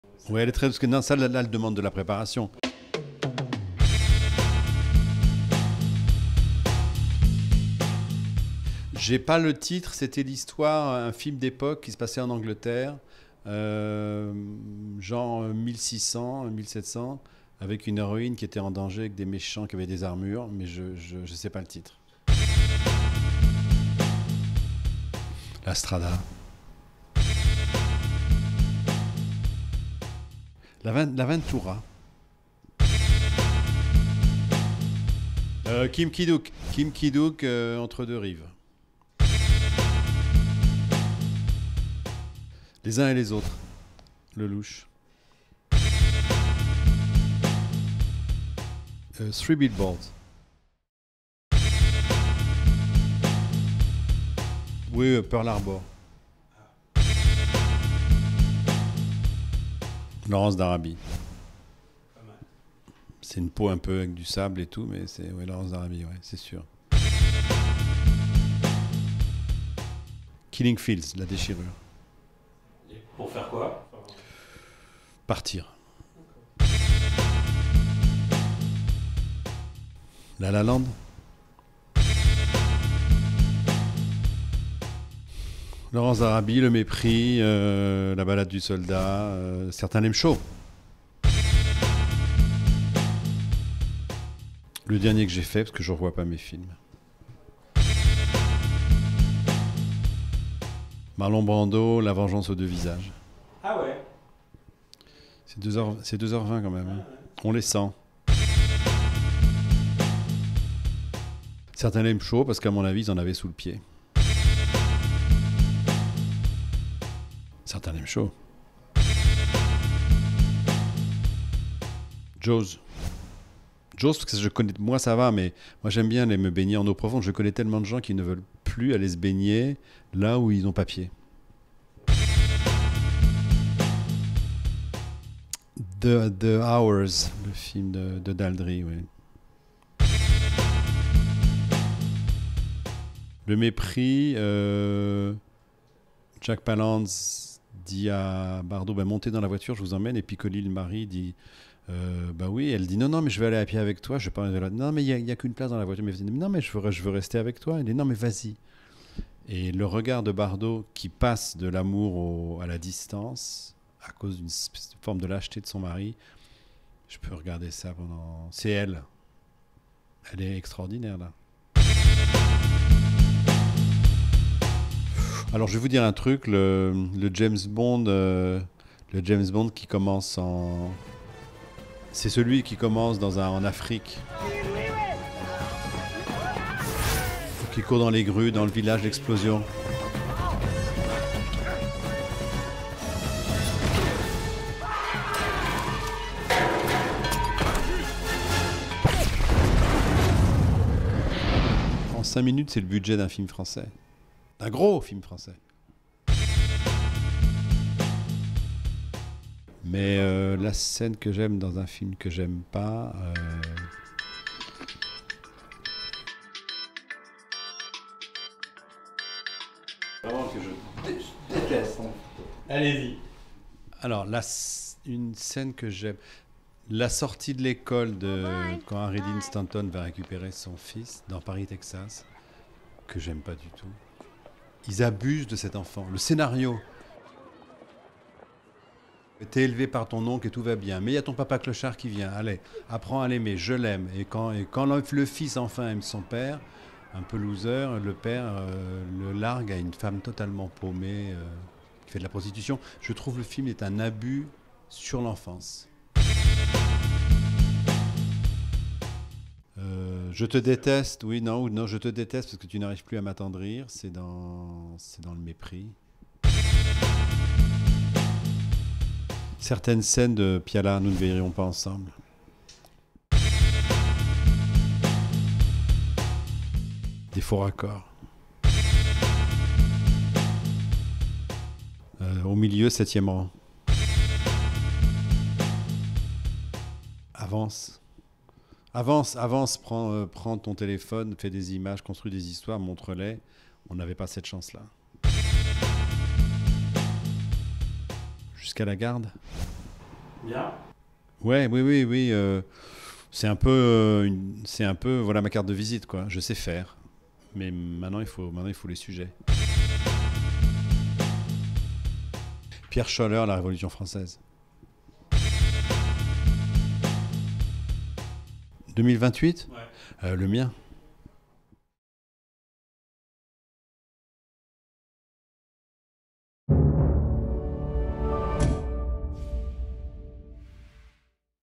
Quiz cinéma : Régis Wargnier
Le réalisateur d'INDOCHINE, Régis WARGNIER est un élève consciencieux. Il a tenu à répondre à toutes les questions de notre quiz cinéma. Quitte à revenir pour nous donner la réponse manquante. Visiblement, LE MÉPRIS, CERTAINS L'AIMENT CHAUD ou encore LAWRENCE D'ARABIE l'ont particulièrement marqué.